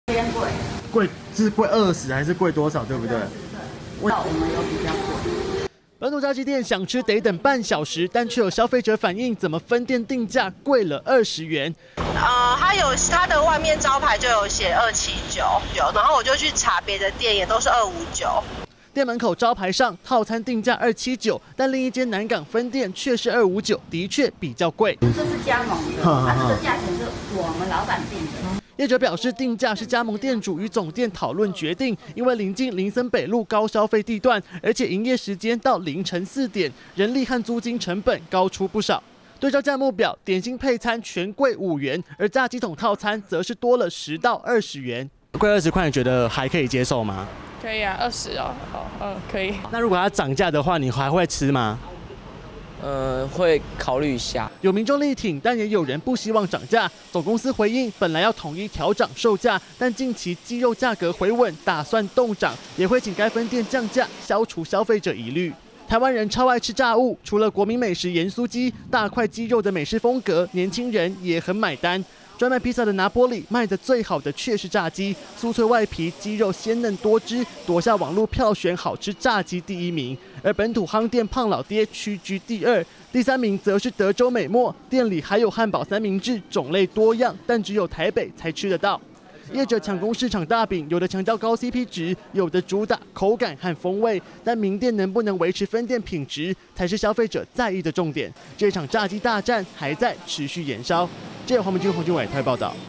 20170829中天新聞 本土炸雞連鎖店不同價 買套餐貴20元
台灣人超愛吃炸物，有本土美式炸雞業者近來爆紅，但卻被民眾發現，有間台北市分店定價貴了5到20元，一份炸雞全家餐其他地方只賣259，但到這間分店卻是要賣279，引發質疑明明是同樣的連鎖店，怎麼會出現不同價格。